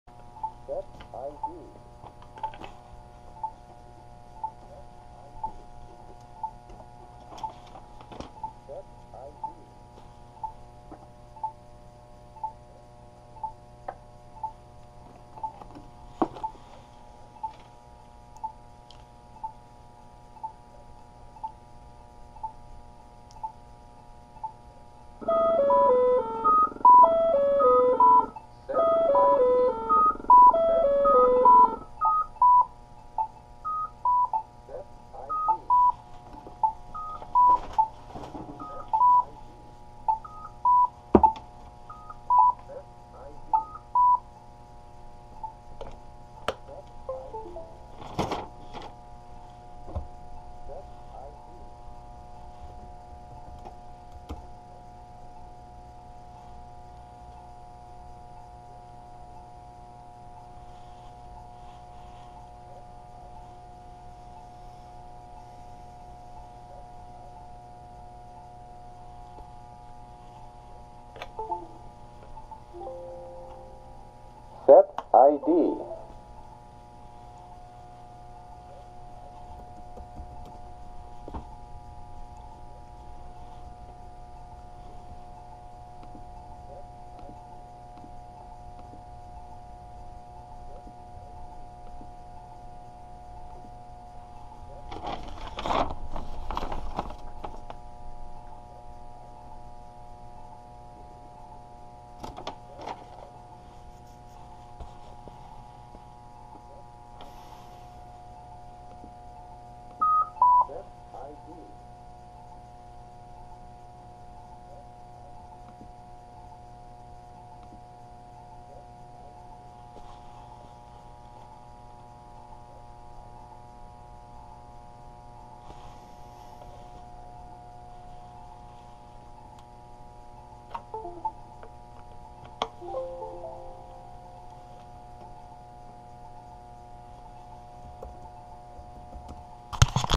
Setting Up ID's on Polara's EN2 Legacy Navigators
In this video, I'm setting up a couple of the individual pushbutton ID's so the CCU (central control unit) will recognize them in the system. The city provided me with a template so I can set the ID's of the pushbuttons for their specific locations at the intersection. Once the button ID is set, the button will tell you to push the button on the back of the CCU so the changes take effect.
These Navigator's are getting installed at a new intersection that our company is working on. They have basic alterations from the factory but have to be programmed, ID's set and have their specific sounds set for North-South & East-West directions.